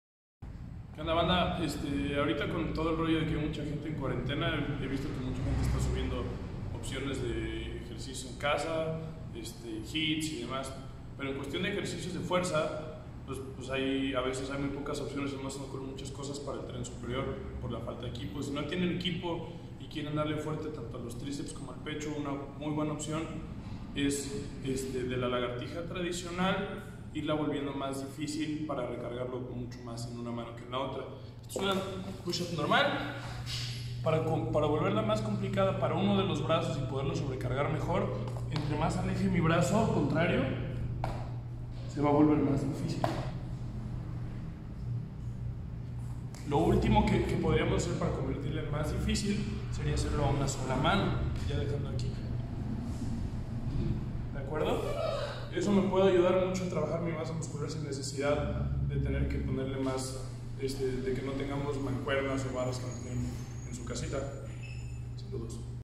Haz más difíciles las lagartijas (pushups)
Como ir progresando hasta llegar a hacer lagartijas a una mano, ideal para entrenar en casa